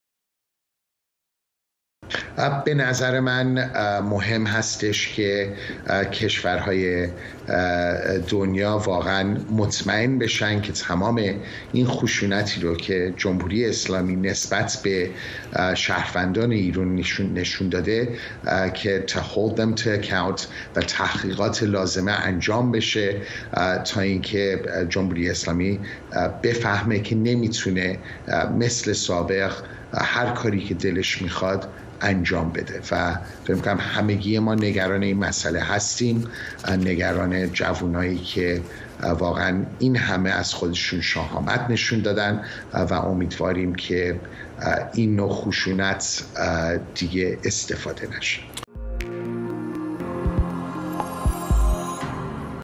علی احساسی: دنیا باید مطمئن شود خشونت جمهوری اسلامی علیه مردم ایران بی‌پاسخ نماند
علی  احساسی، رئیس کمیته امور خارجی و توسعه بین الملل پارلمان کانادا: دنیا باید مطمئن شود خشونت جمهوری اسلامی علیه مردم ایران بی‌پاسخ نماند